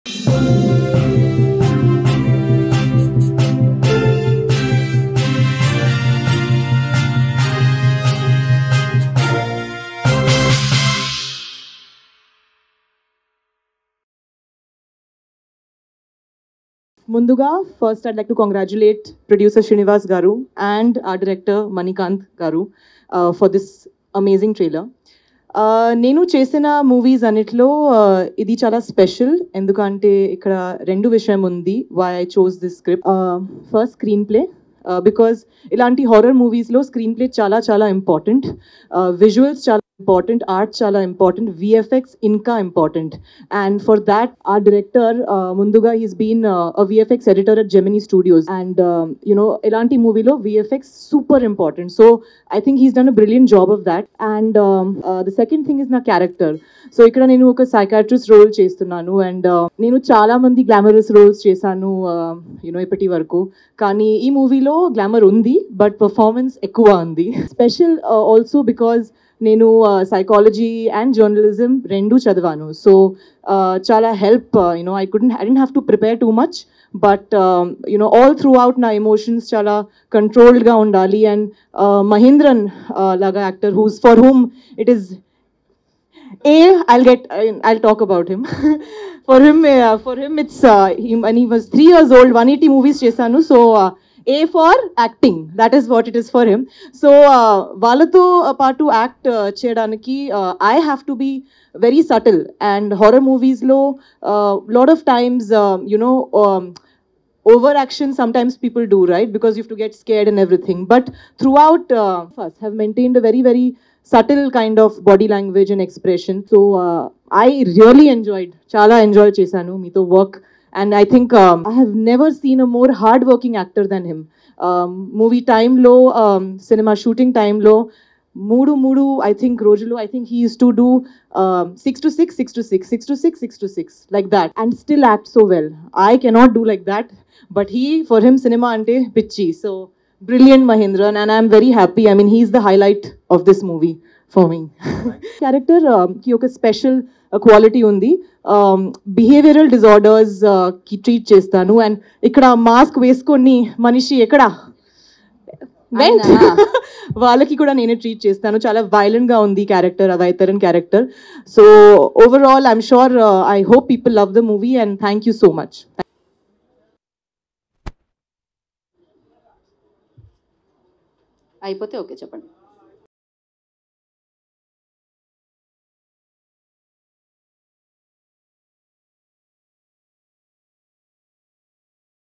Actress Shraddha Das Speech About Her Role in Arrtham Movie | Arrtham Movie Teaser Launch Event
Actress Shraddha Das Speech About Her Role in Arrtham Movie | Arrtham Movie Teaser Launch Event || SocialPostTv

Hello Friends,